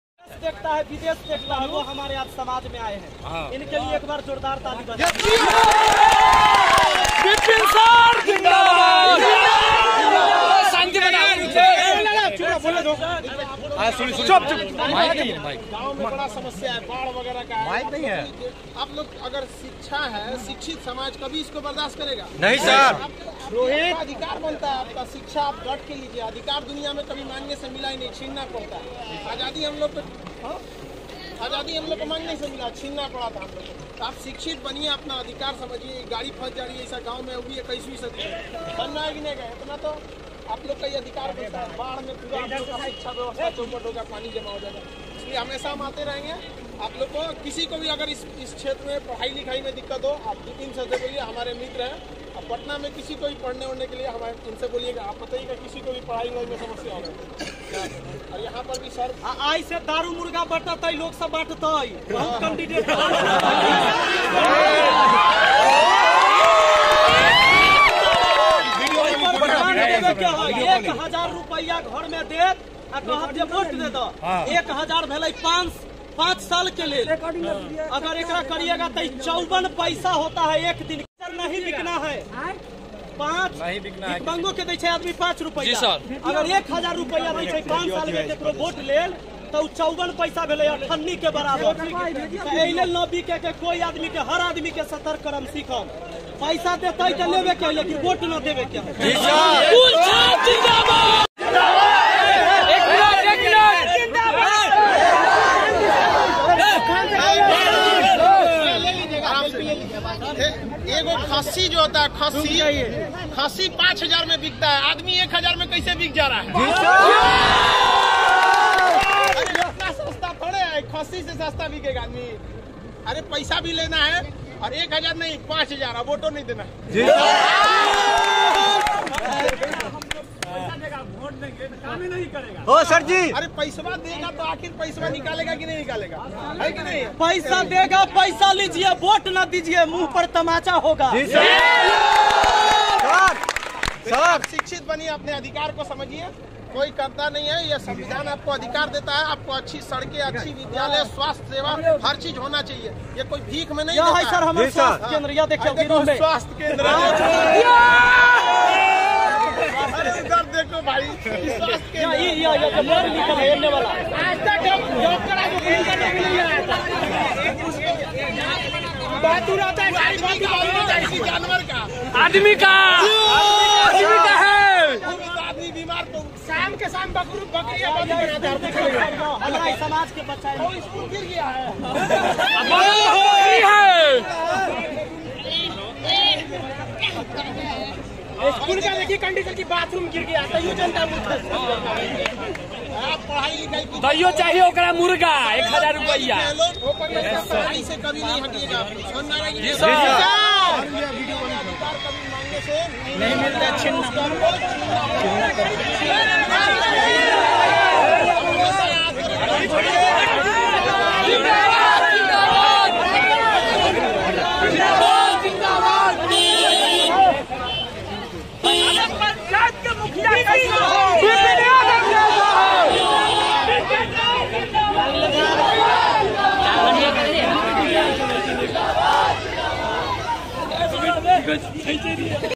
मुखिया प्रत्यासी Math Masti Vipin Sir का चुनाव प्रचार Khan Sir के अंदाज में
science by priya mam